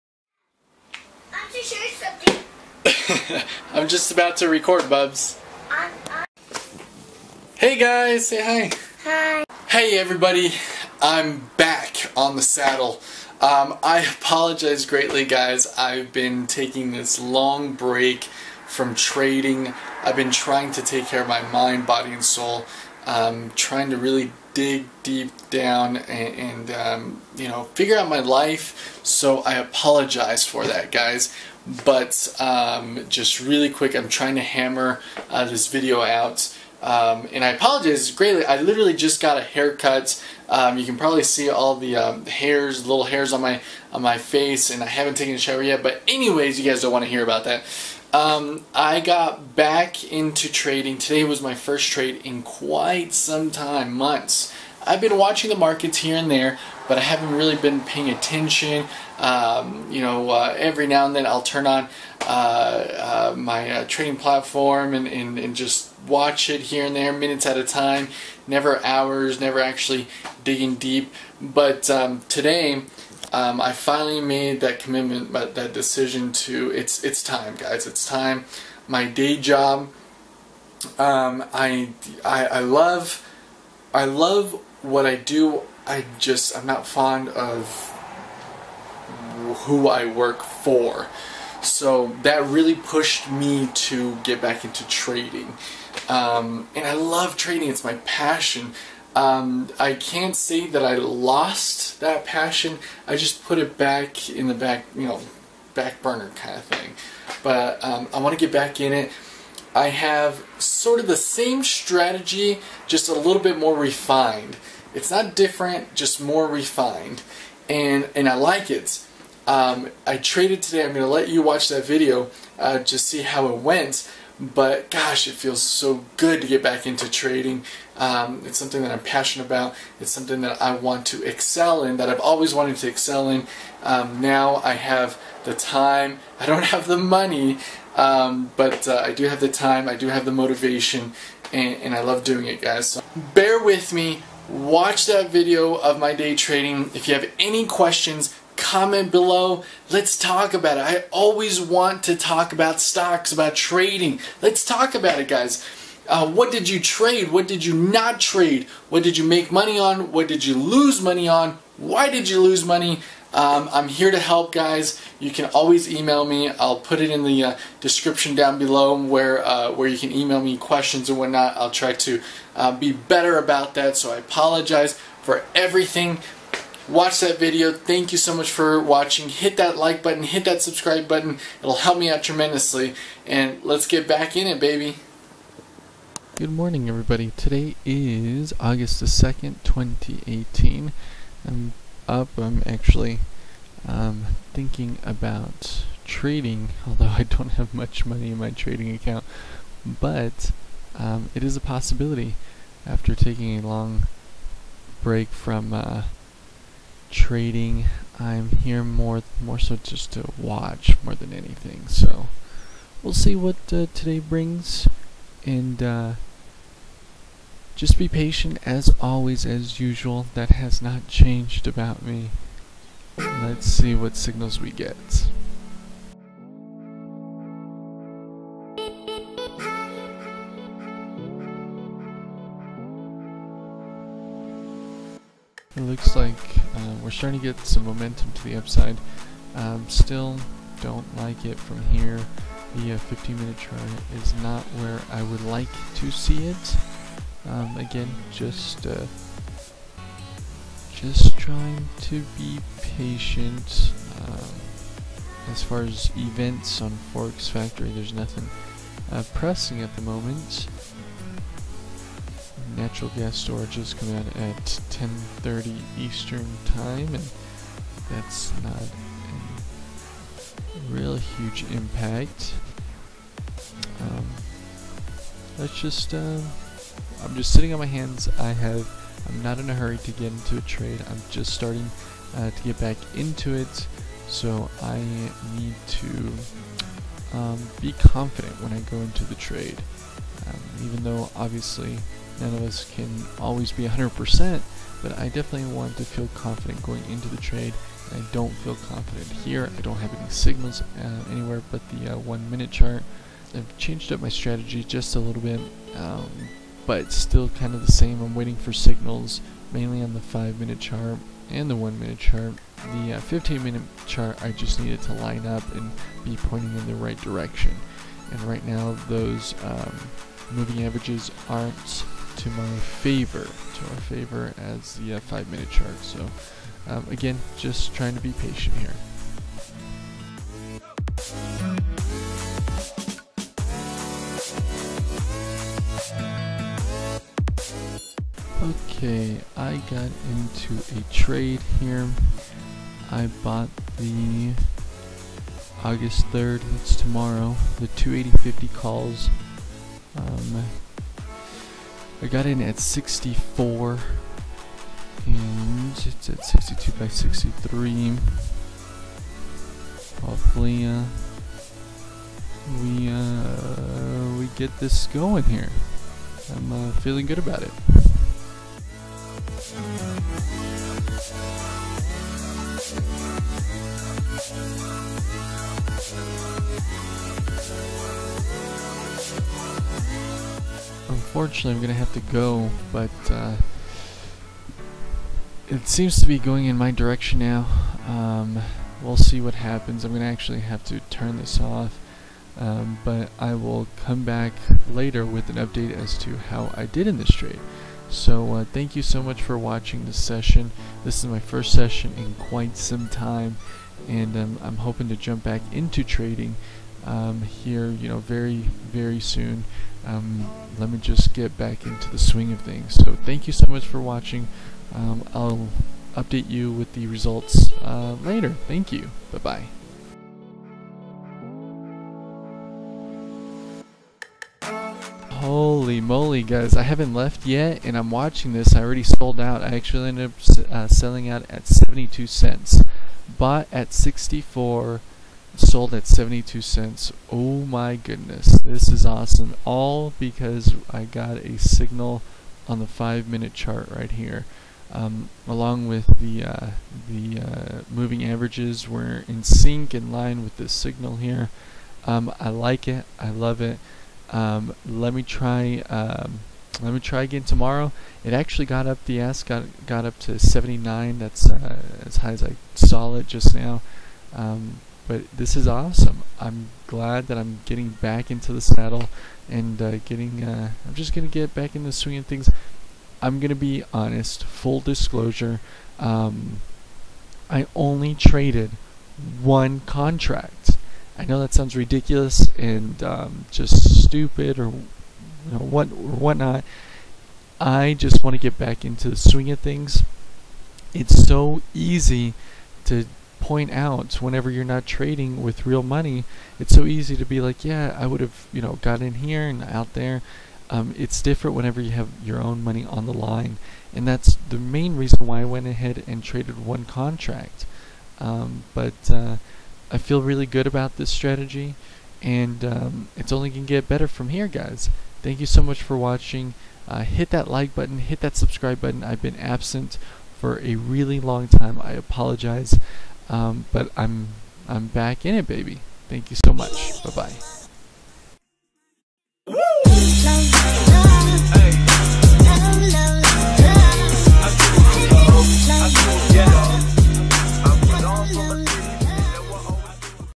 First Day Trade In Months....What Do You Think Happened?!?!
Day Trading Live August 2, 2018. First day trade in months and I am back at it again! Please let me know what you think of my videos. Thank you so much. LETS MAKE SOME MONEY!!!